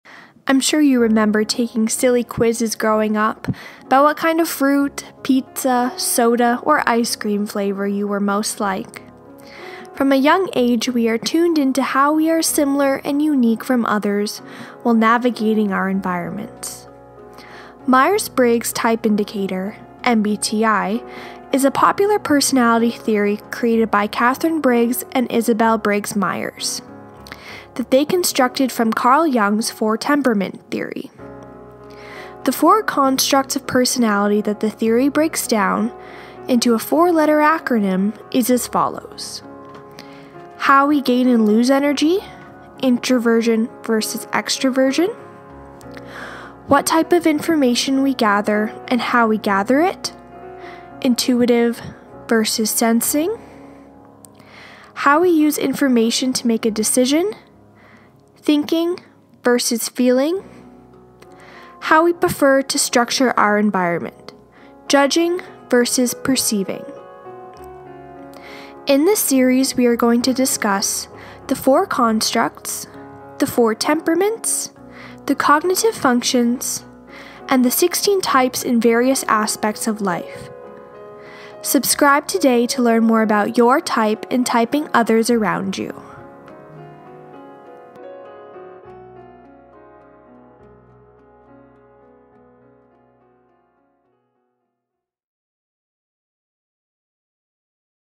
MBTI Series: Introduction
First look at Myers Briggs Theory